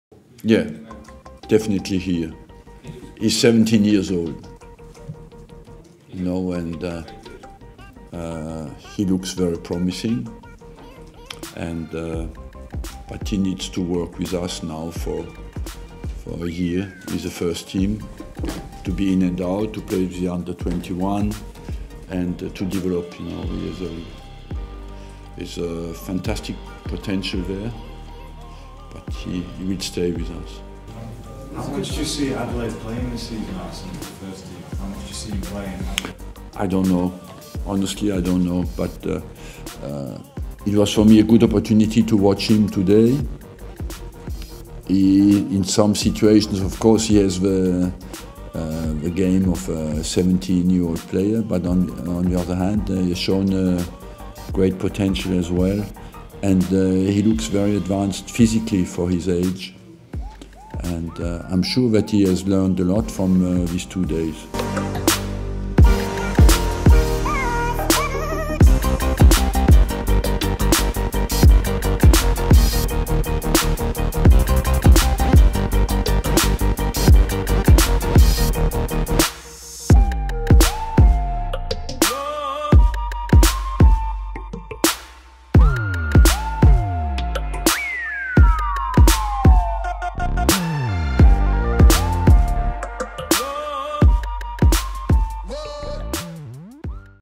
Jeff Reine-Adelaide | Emirates Cup 2015 | (With Wenger's Comments)
Wenger described Jeff Reine-Adelaide as a 'special talent' after the exciting 17 year old prospect performed very well in the Emirates Cup.